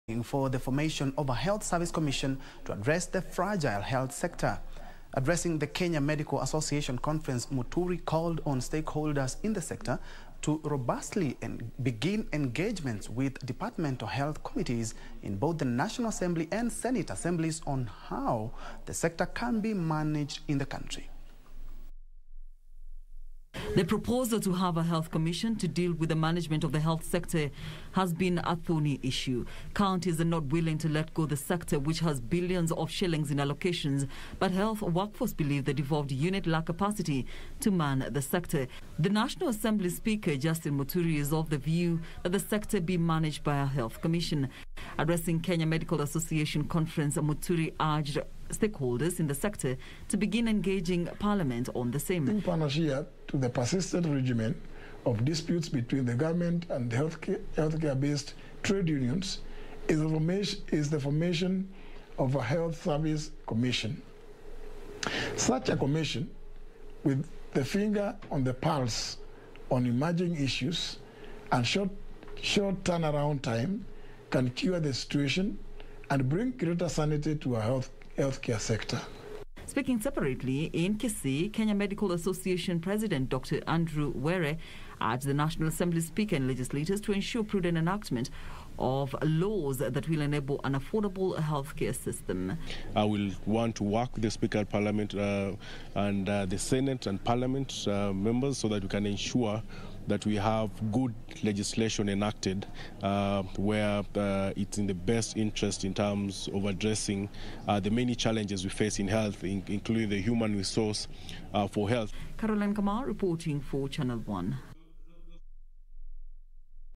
Speaker Muturi advocates for the formation of a Health Service Commission
The Speaker of the National Assembly Justin Muturi is advocating for the formation of a Health Service Commission to address the fragile health sector. Addressing the Kenya Medical Association conference Muturi called on stakeholders in the sector to robustly begin engagements with departmental health committees in both the National Assembly and Senate assemblies and how the sector can be managed in the country.